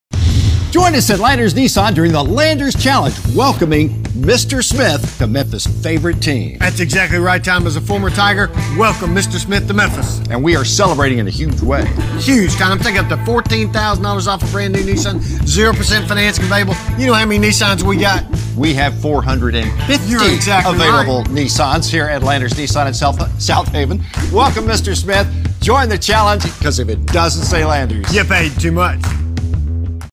Landers Nissan Welcomes Mr. Smith to Memphis
Here at Landers Nissan we want to welcome Mr. Smith to the Tigers, Memphis' favorite team. We are celebrating with up to $14,000 off a New Nissan and 0% financing available with over 450 vehicles to choose from! Join the celebration and come see us LandersMemphis.com